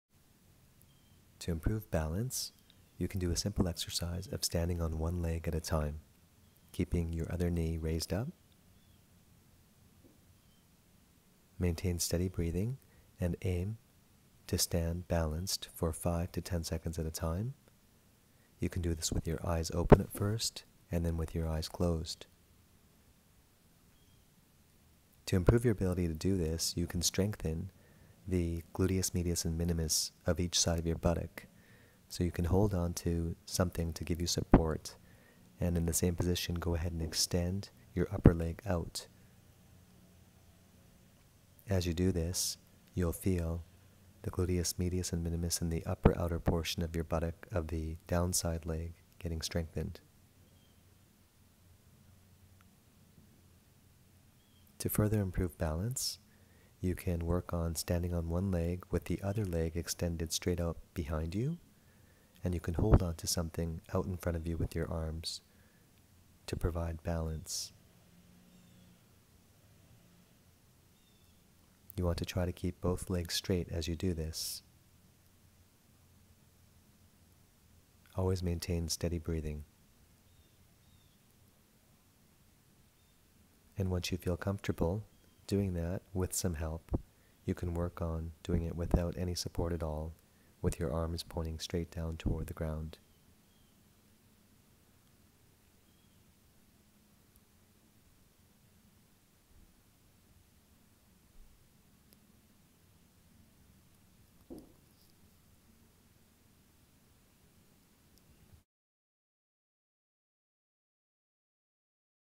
Simple Exercises to Improve Your Balance
To begin, stand on one leg at a time and try to maintain your balance for as long as you comfortably can, first with your eyes open, and then with your eyes closed.

Next, straighten your up-side leg straight out in front of you for as many repetitions as you can comfortably manage - you can hold onto something with your hands to help you stay balanced as you do this.  Strive to keep your down-side leg straight - this will help develop strength in the hip abductors of your down-side leg, called your gluteus medius and minimus - these are the muscles that are primarily responsible for keeping you balanced as you stand, walk, and run.  You will feel these muscles get a good work out along the upper and outer aspect of the buttock of your down-side leg.

Next, balance yourself on one leg with your up-side leg extended straight behind you as high as you can comfortably take it.  You can support your hands on something in front of you like a chair, table, or gym equipment.  Repeat on the other side.

Then, try the same balancing exercise without support for your arms.  You can keep your arms pointed down toward the ground or straight out above your head.

For an overview of exercise progressions that you can work at to improve your mobility and balance in a systematic way, please feel free to visit our Mobility Exercise Progressions page here:

For some suggestions on how to set up a simple workout area at home, please feel free to view: